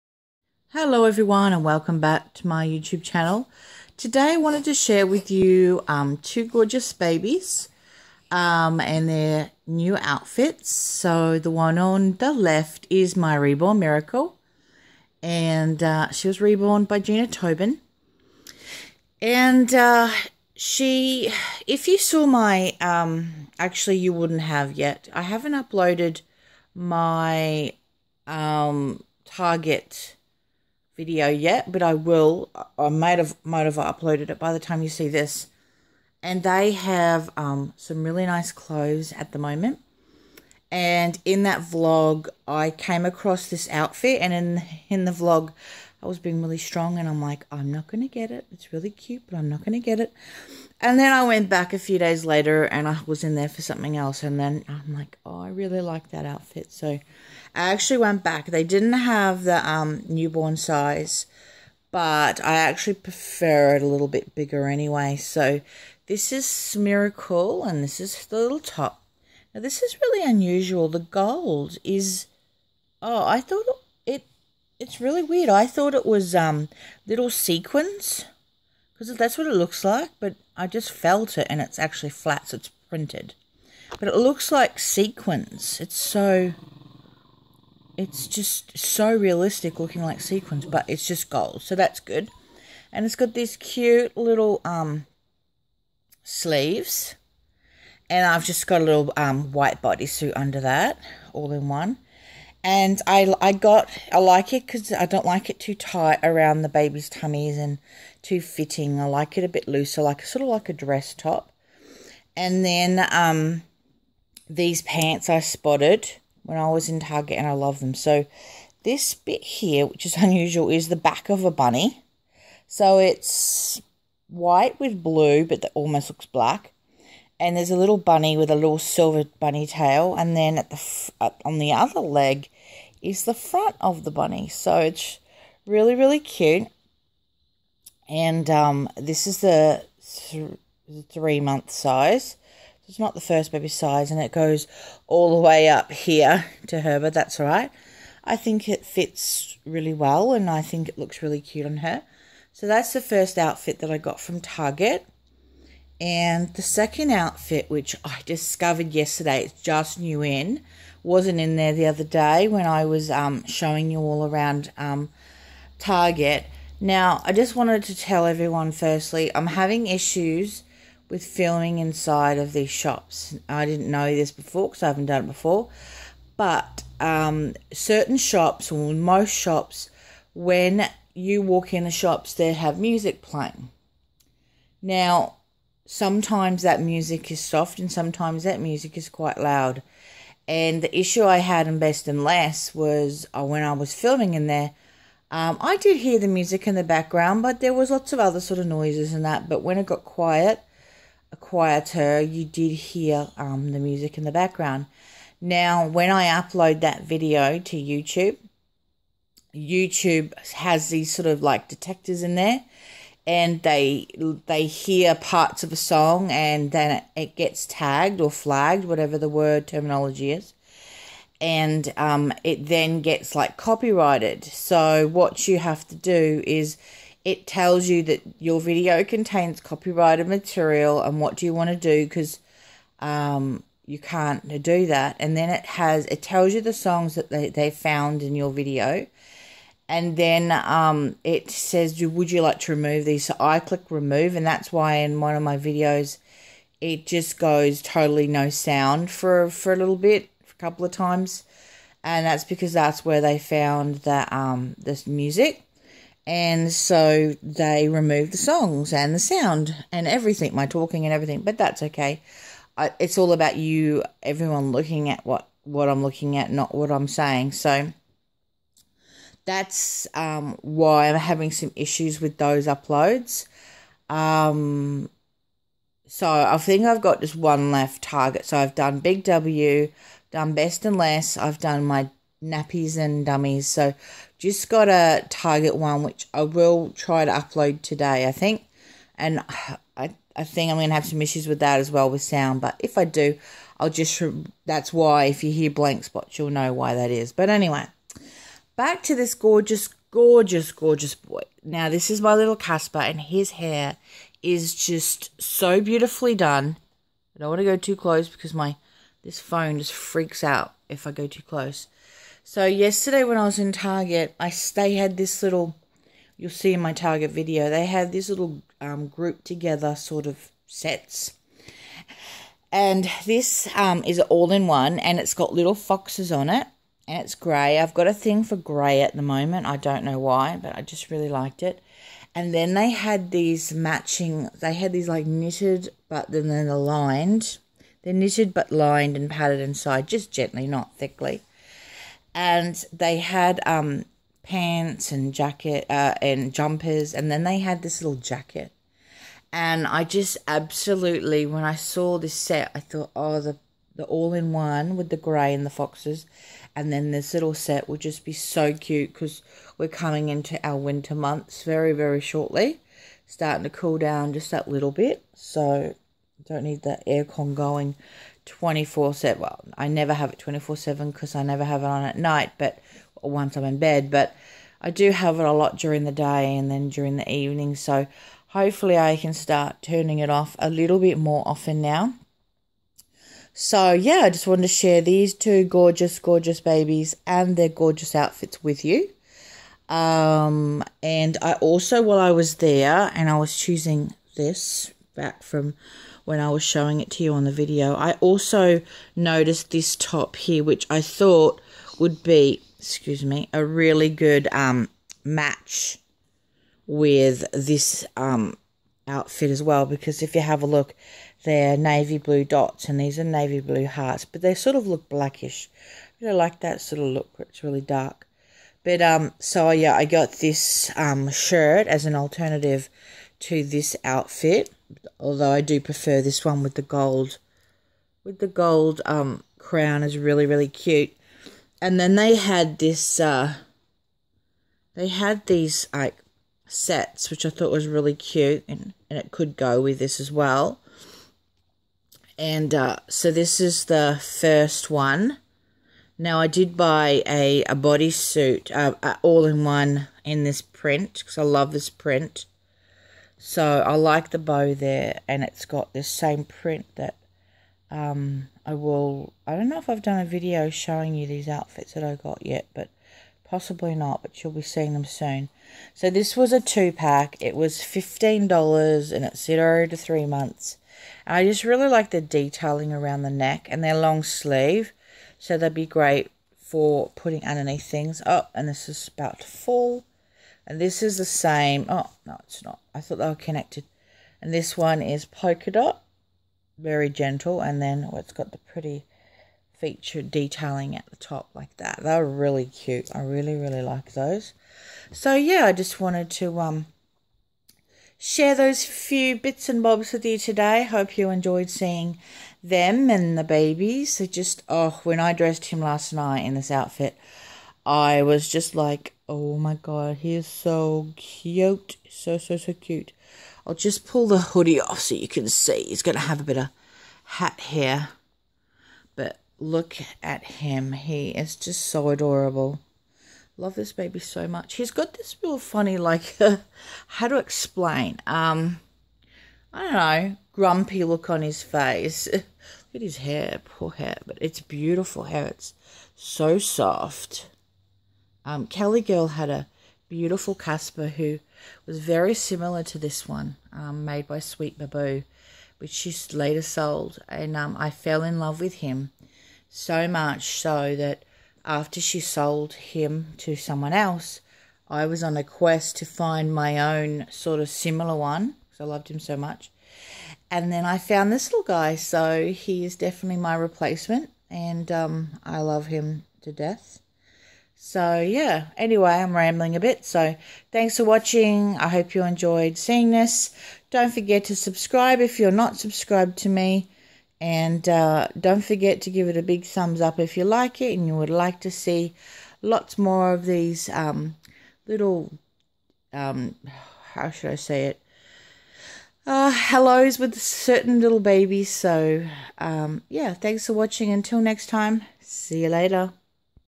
Christina'sReborns - Reborn Miracle and Caspar wanted to say hi!
Caspar and Miracle wearing their new outfits from Target!